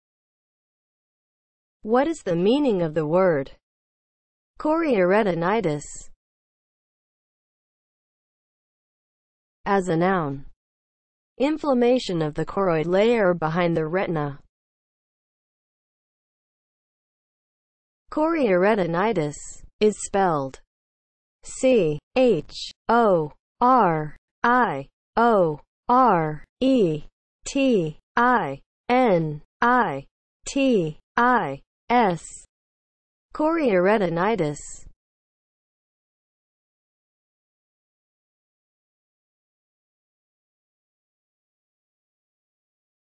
What is the meaning of the word CHORIORETINITIS?
Definition and spelling of the word CHORIORETINITIS.

(00:00) Intro
(00:08) As a noun
(00:16) Spelling

As a noun (00:08):
• inflammation of the choroid layer behind the retina